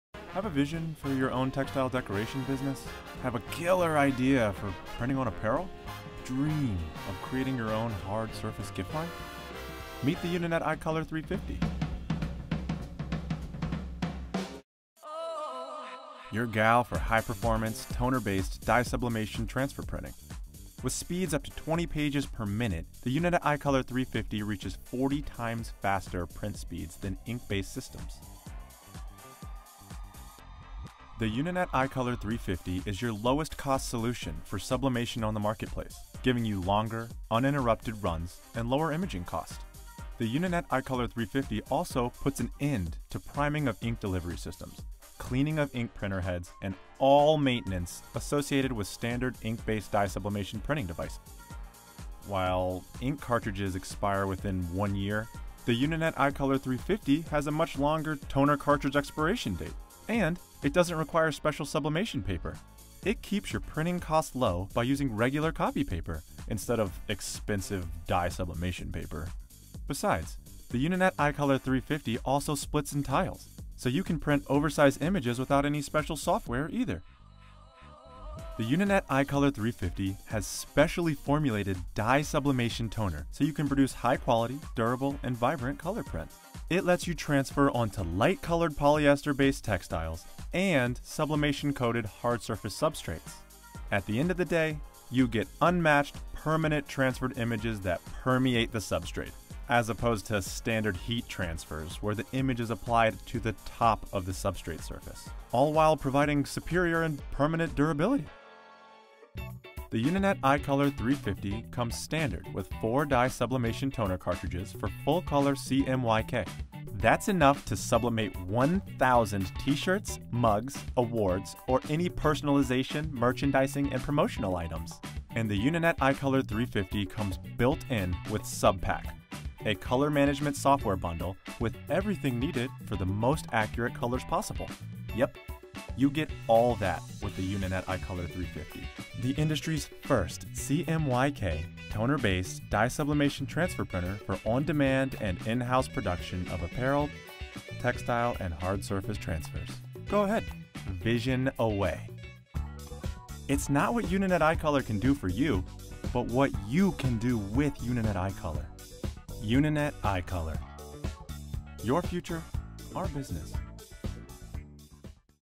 UNINET IColor 350 Toner Dye Sublimation Transfer Printer
The IColor 350 is an on-demand, full color toner-based dye sublimation transfer printer for short to mid run dye sublimation transfer production of garments, hard surfaces, and much more! The IColor 350 does not require special software or special transfer media. Keep costs low by printing on regular copy paper instead of expensive dye sublimation media.

UniNet reinvents dye sublimation with the IColor 350, the industry’s first of its kind toner-based Dye Sublimation Transfer Printer. The IColor 350’s dedicated platform for dye sublimation transfers has been optimized for top quality printing, high performance, and superior reliability in a tabletop-sized footprint.

Unlock the Power of Toner-Based Dye Sublimation
The IColor 350 incorporates its specially formulated dye sublimation toner technology for production of high quality, durable and vibrant color prints. This system uses heat to transfer images in a one-step process onto white or light-colored polyester-based textiles and sublimation-coated hard surface substrates.

The toner formulation was smartly developed to serve as a carrier for the sublimation process, which transitions from a solid to a gas state. The result delivers cost-efficient, permanent transferred images that permeate the substrate, versus standard heat transfers where the image is applied to the top of the substrate surface.

The IColor 350 printing system puts an end to priming of ink-delivery systems, cleaning of ink printer heads, and all maintenance associated with standard ink-based dye sublimation printing devices. It is also uniquely suited to ensure higher yields and prints at much higher speeds than competing ink-sublimation systems, delivering greater efficiency and productivity.

Enhance Your Apparel and Hard Surface Decoration Business
The UniNet IColor 350 offers on-demand and high-performance printing capabilities for dye sublimation transfers to transform a plethora of polyester-based textiles and sublimation coated hard surface substrates such as ceramic, metal, acrylic, UniSub, DynaSub, SubliBoard and more. Create a wide variety of garments, cell phone covers, awards, flip-flops, bags, coasters, ceramic mugs, home decor and more. For best results we suggest pressing transfers with a Hotronix Fusion or Geo Knight heat press.

What's so unique about the IColor 350 dye sublimation printer compared to ink based systems?

 Suited for production level environment, the IColor 350 features vastly faster print speeds: 20 pages per minute vs 1 pager per 2 minutes for ink
No clogged heads resulting from non-use. Toner based systems do not have this maintenance issue
Much longer expiration date on toner (ink expires within one year). Uninet suggest the cartridges are used within a 2 year period.
Much higher page yield options
The ability to use regular copy paper rather than specific dye sub media
IColor 350 Offers Sublimation Without Limitations
No printer maintenance required: No clogging or printhead cleaning resulting from non-use like ink-based systems; toner-based systems do not have this maintenance issue
Provides superior durability – over 150 wash cycles
Toner-based dye sublimation delivers softer transfers for garment decoration
Ensures significantly extended yields and lower imaging costs
Features significantly faster print speeds: 20 pages per minute vs. 1 page per 2 minutes for ink (photo mode), suited for production level environments
Toner-based dye sublimation produces better pastels and gradients with a wider color gamut for vibrant and true-to-life color reproduction
Keep printing costs low by using regular copy paper instead of expensive dye sublimation media
Does not require special software
Much longer expiration date on toner; ink expires within one year
Included ICC color profile for the most accurate colors possible
UniNET IColor 350 Sublimation Printer Incudes:

UniNet iColor 350 Printer
Cyan, Magenta, Yellow, and Black Toner Cartridges
Driver
Instruction Manual
Toner Cleaning Cloth
Network Cable
Power & USB Cords
1 Year Warranty
$500 in So Fontsy Designs - SVGs, JPGs, Commercial Use (Please note that the card is valid for the card redemption portion of the site)